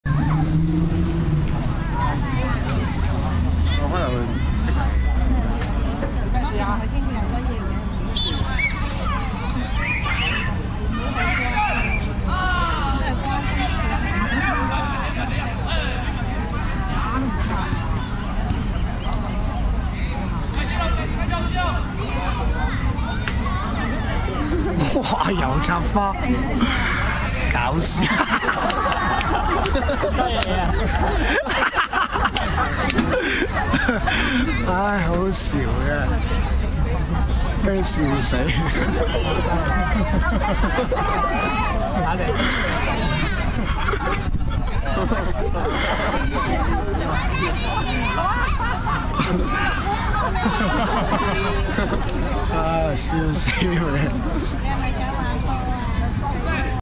hong kong ronaldo
little kid playing a amazing football